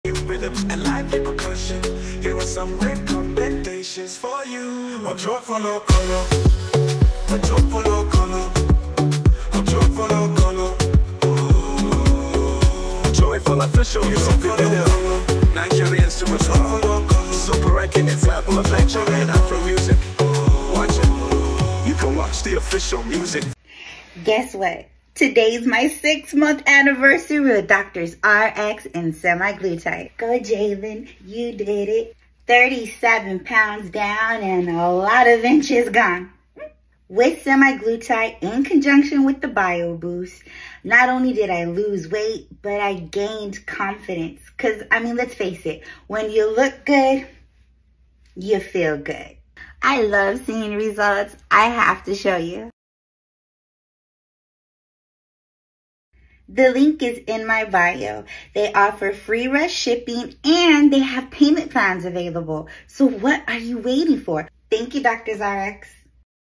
Jaylynn Juelz - Brand Ambassador, Plus-Size Model | Celebrating 6 Months with Semiglutide!
Join me as I celebrate 6 incredible months on my journey with Semiglutide! 🌟 As a proud brand ambassador, content creator, and plus-size model, I’ve experienced amazing benefits with Semiglutide, including a free Bio Boost to enhance your overall wellness.

Discover the many perks that come with Semiglutide, from improved energy to a healthier lifestyle. I’m here to share my journey, motivate others, and embrace the beauty of being a BBW.

Follow us today and be part of this life-changing experience!